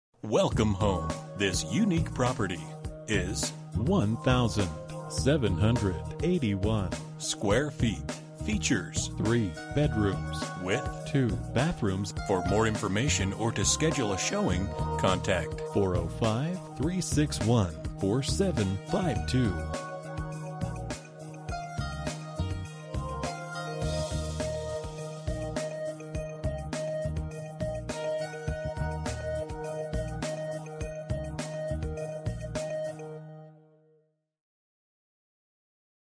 Residential for sale - 1425 NW 17 Place, Newcastle, OK 73065
This home lives much larger then its square footage on the plans.  Absolutely no wasted space in this 3 bed, 2 bath, 3 car garage home built in the quiet community of Meadow Creek 2.  Minutes from all amenities OKC, Moore, and Newcastle have to offer.  Home features 3 cm granite, tile and carpet flooring and a front and rear covered patio. Kitchen cabinets feature soft close hinges on the doors and full extension drawers.  Act quickly and have time to pick finishing details.

Bedrooms: 3
Bathrooms: 2
Square Feet: 1781
Price: $249,900